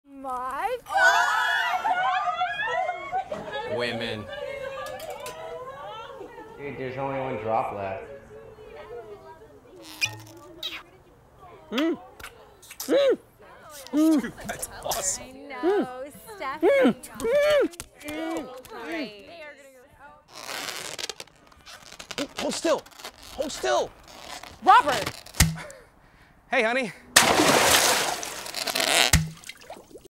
Heinz - Last Drop
A guy finds himself stuck at a baby shower, in more ways than one.